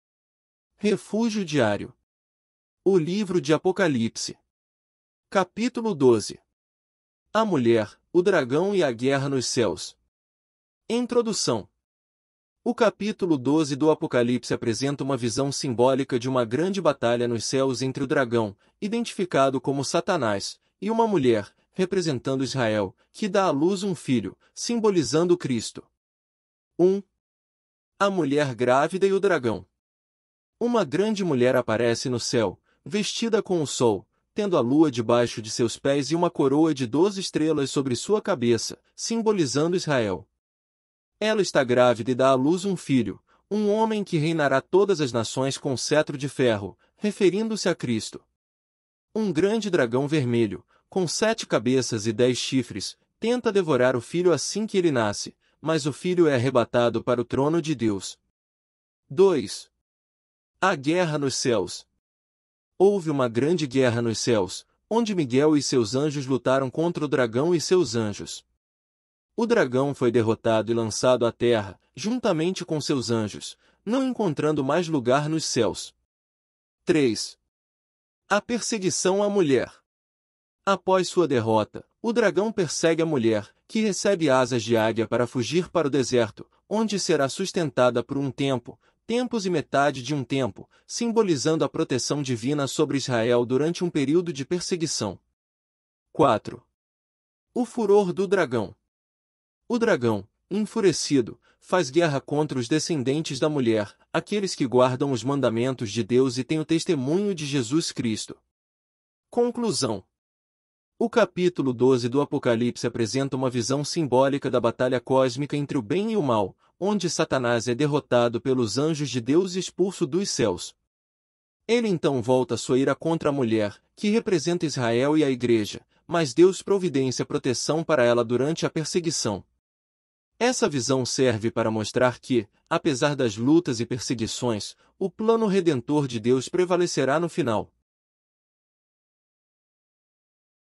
Refúgio Diário - APOCALIPSE - Capítulo 12: A Mulher, o Dragão e a Guerra nos Céus
Introdução:
O capítulo 12 do Apocalipse apresenta uma visão simbólica de uma grande batalha nos céus entre o Dragão, identificado como Satanás, e uma mulher, representando Israel, que dá à luz um filho, simbolizando Cristo.